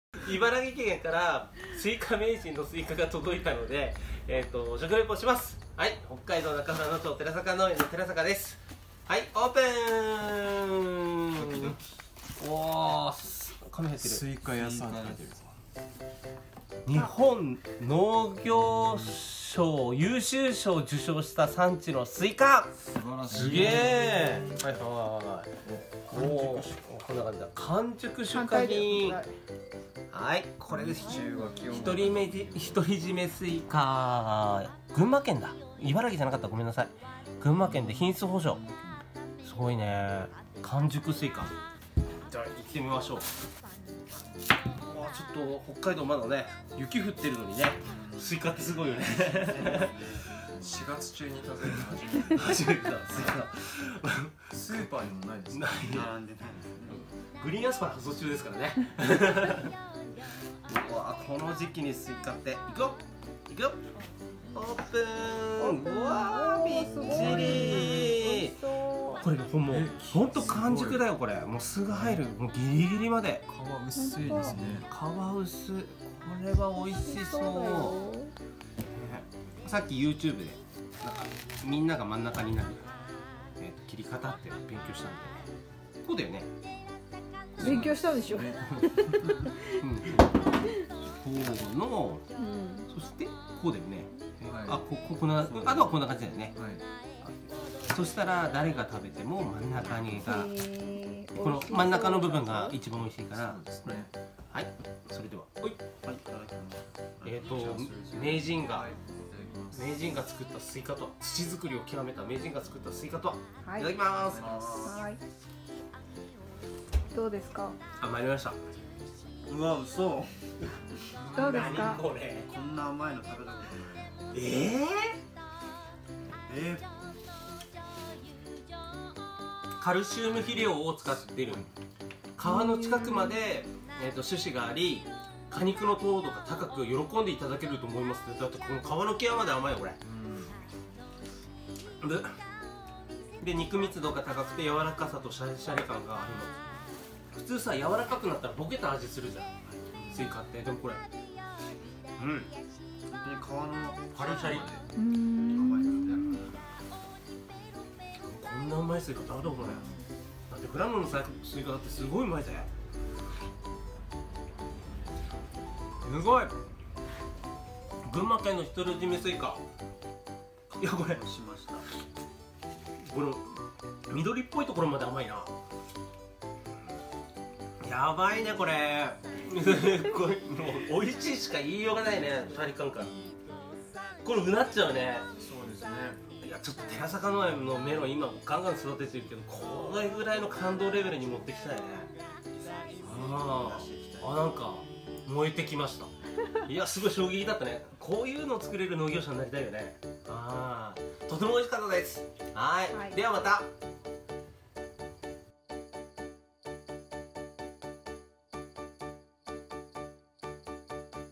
スイカ名人の旨いおいしいと感激：北海道 富良野 メロン農家の寺坂農園
感激レベルのおいしさっ！🤩
さすが、日本農業賞受賞・これがスイカ名人おいしい甘さなのかっ！学びの食レポ動画です